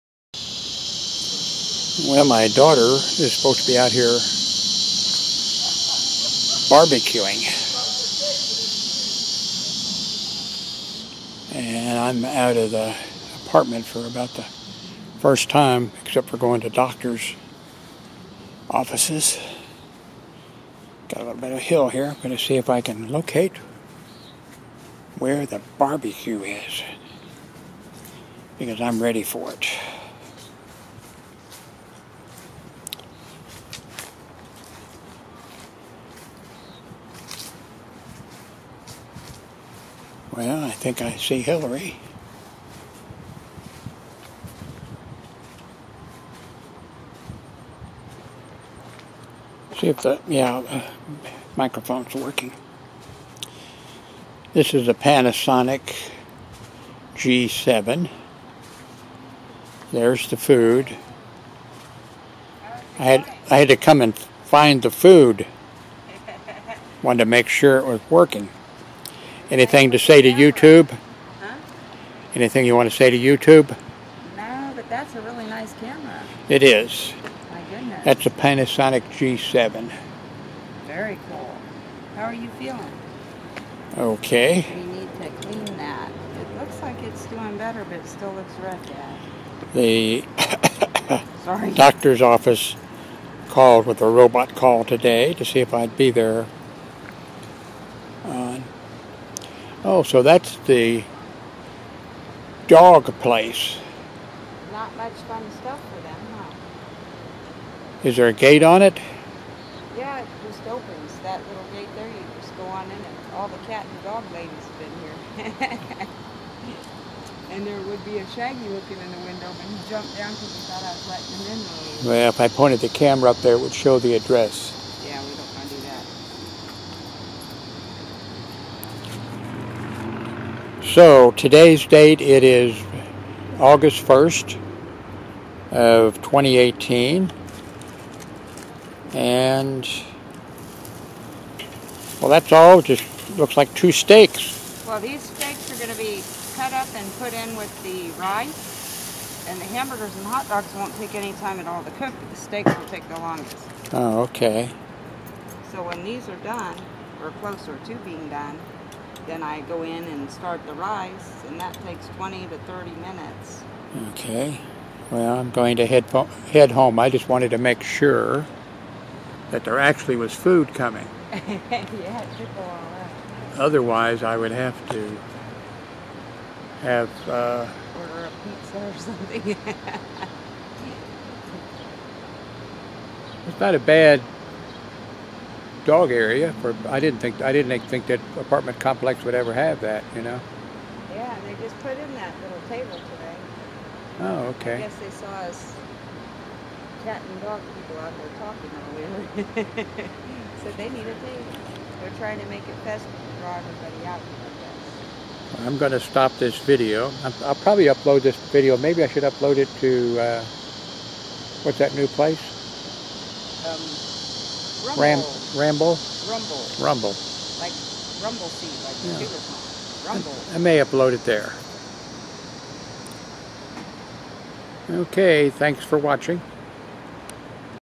Jim looking for food on August 1st of 2018
Hillary, my daughter, said she was grilling food for us.  I went out to check and make sure we had food on the way.
I found Hillary and the food.